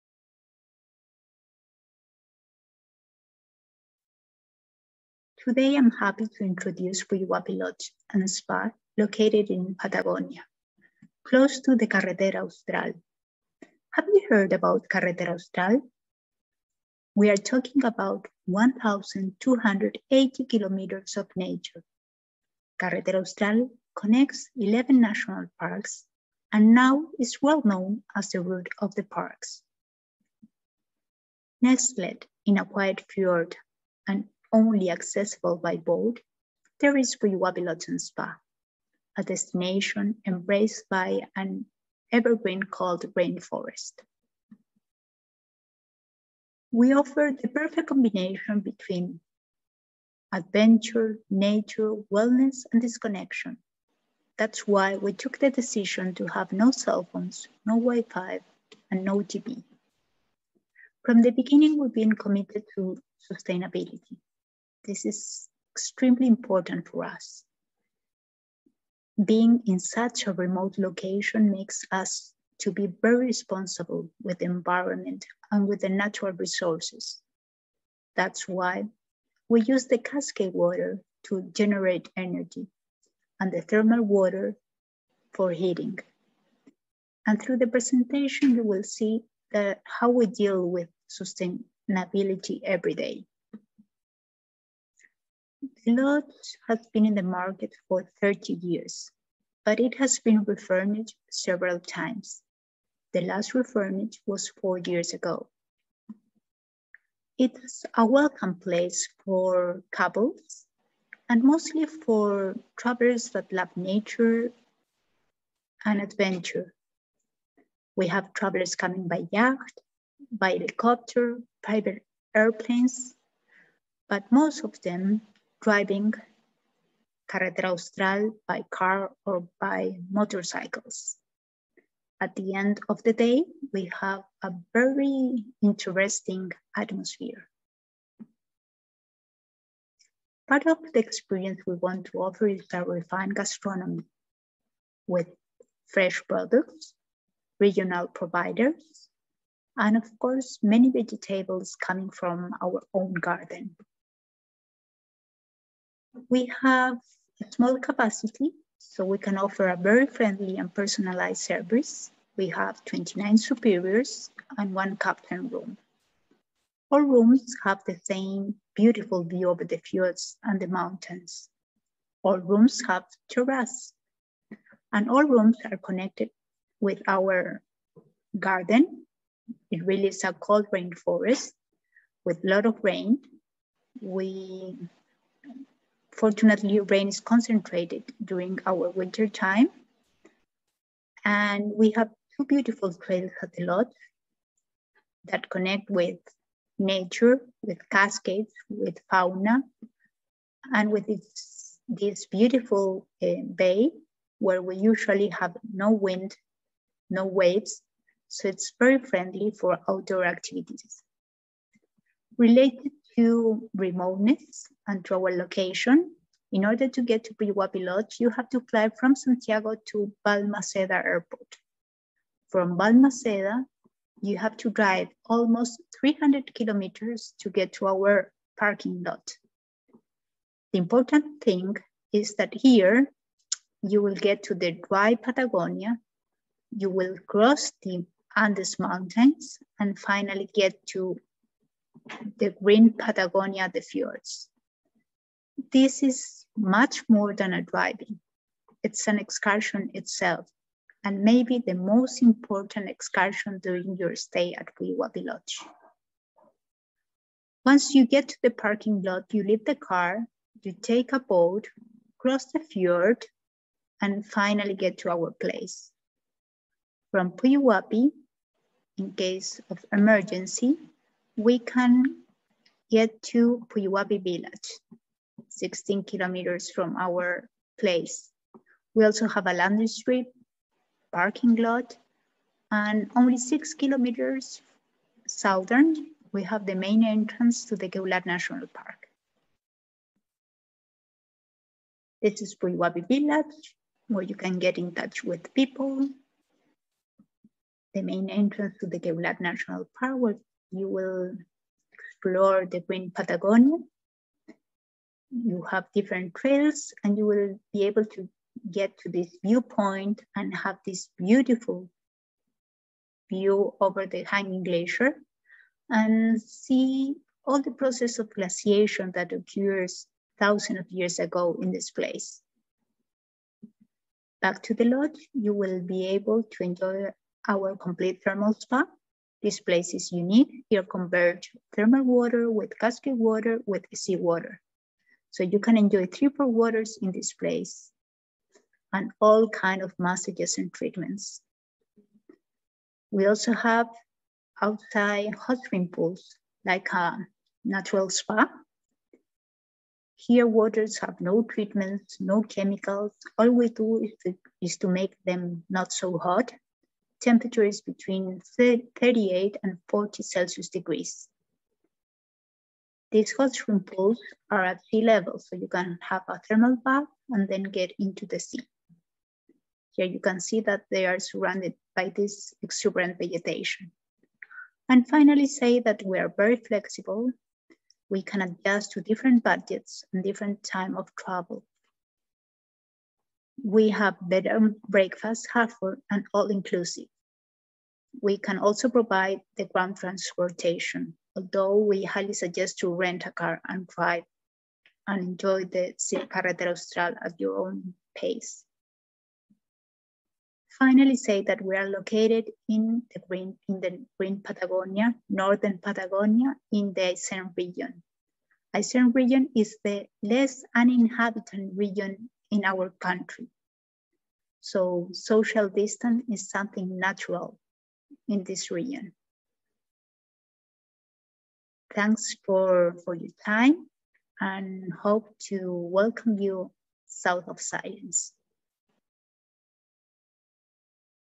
Puyuhuapi Lodge & Spa
Chile has a long stretch of land, which is well complemented by excellent roads that invite you to tour the country on four wheels and enjoy it at your own pace. Where you will be the master of the route and you will have all the freedom to travel new paths and delay as long as you want to enjoy.
Therefore, we invite our entrepreneurs to tell you from the first person why they will be the perfect hosts for your romantic trip.